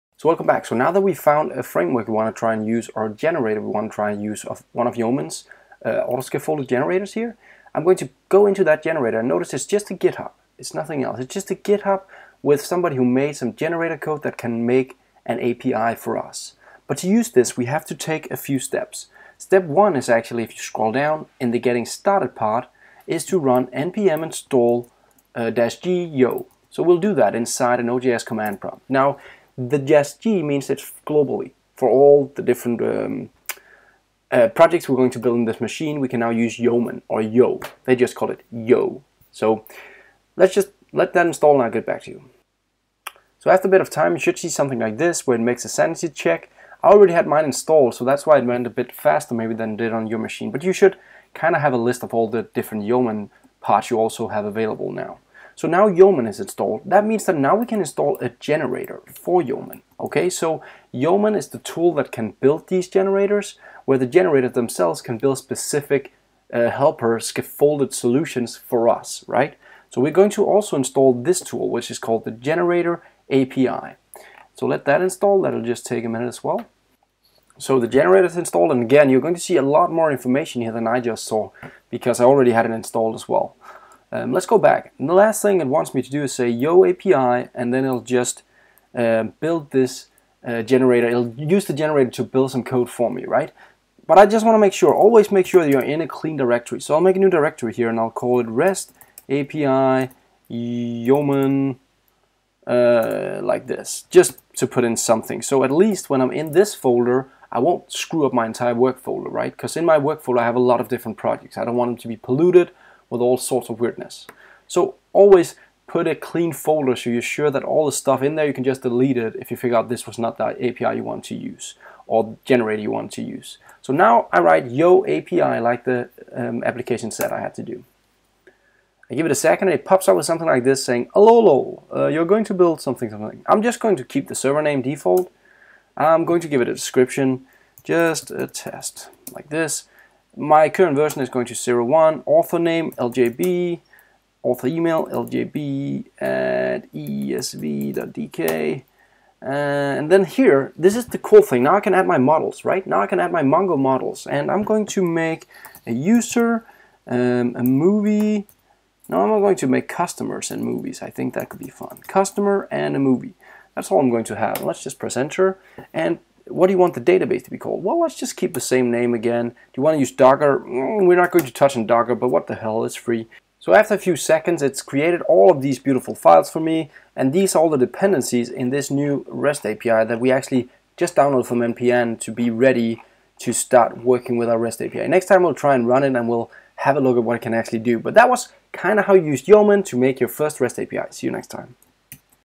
Yeoman first generator install to make a rest api
This Series is about building Full-stack Web Applications in Javascript, using a MongoDB, ExpressJS, NodeJS as a Rest API.
In the next series we will be using a few different frameworks like Aurilia, Angularjs that uses the created REST API to consume and manipulate the data in our Mongodb.

In this, the first part of the Series, the focus will be on explaining what a MEN REST API is all about and learning about the Mongo, ExpressJS and NodeJs